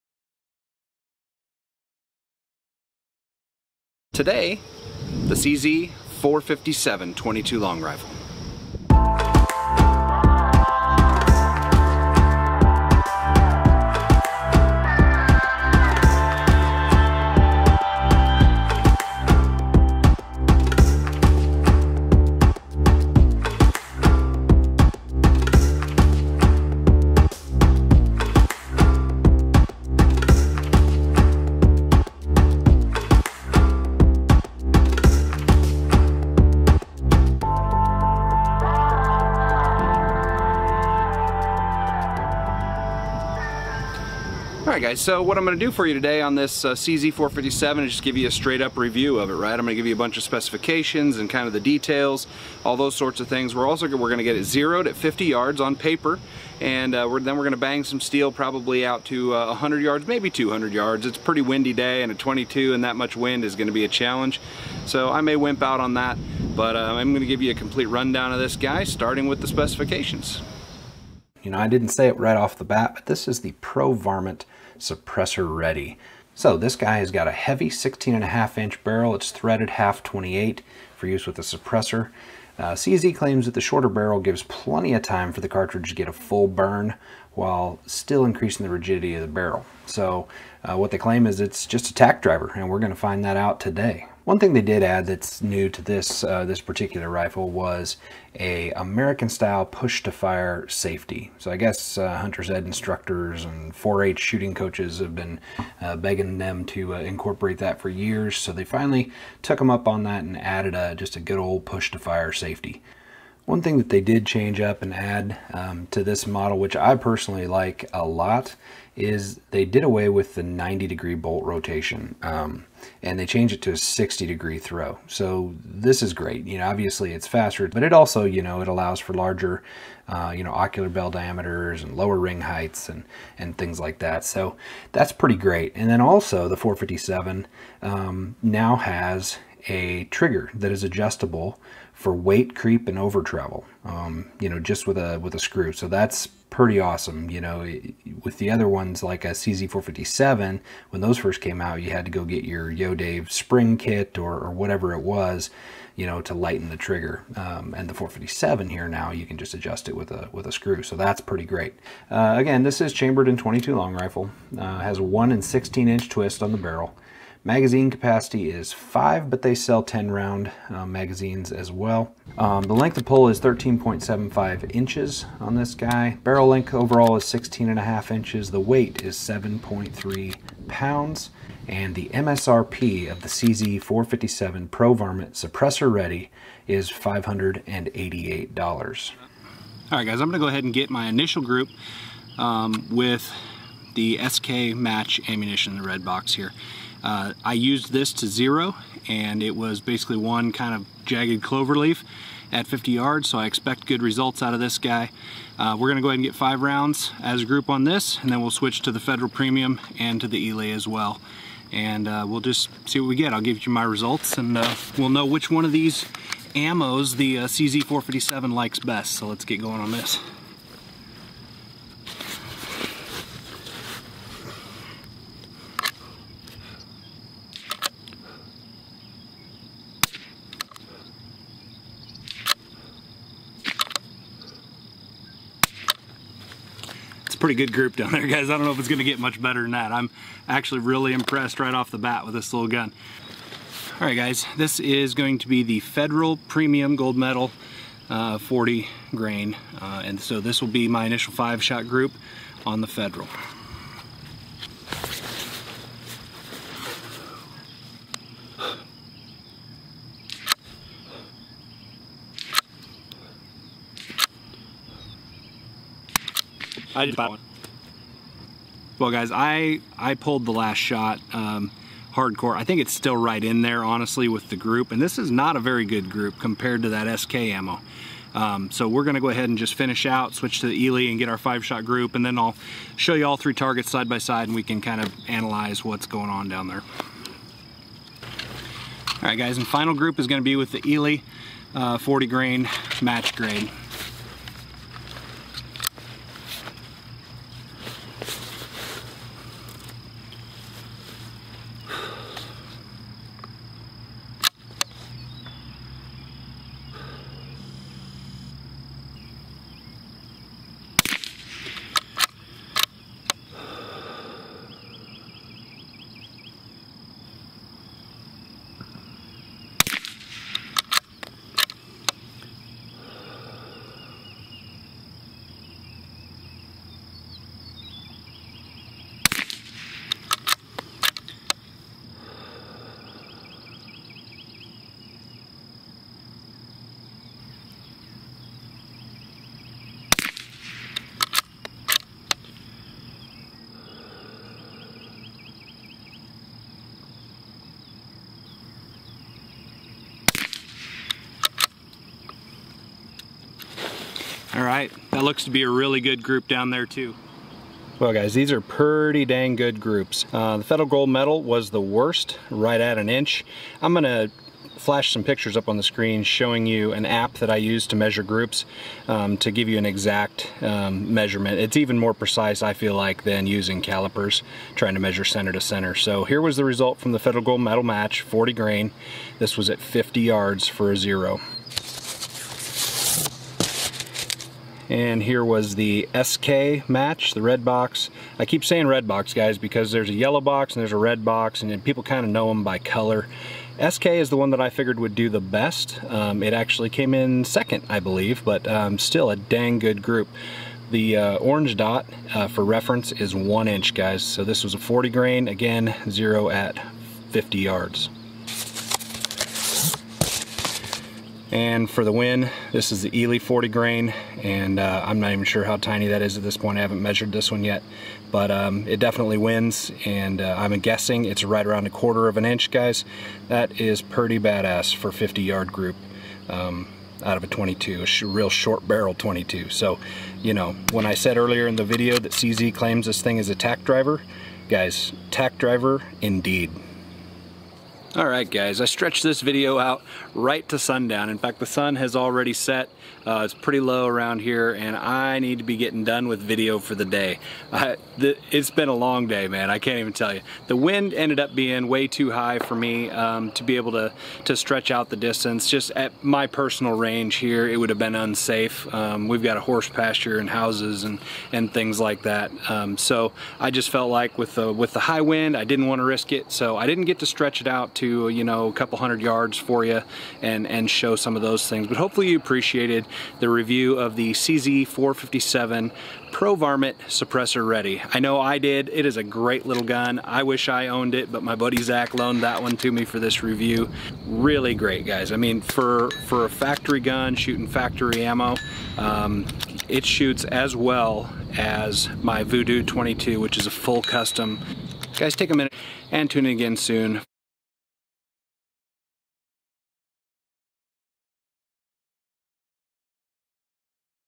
CZ457 Pro Varmint 22LR Review
This is a full overview and range review of the CZ457 Pro Varmint 22LR firearm. This gun is extremely accurate with very good groupings. Let us know if you like this varmint hunter in the comments below and if there is another varmint rifle you would like to see a review of.

Steel Gong Target - 10 x 3/16 inch for 22LR:

Big Dawg Targets 250 Target Roll - 3" Inch Adhesive Splatter Target:

by TFB TV used under CC Attribution license (reuse allowed) - cut and edited from the original.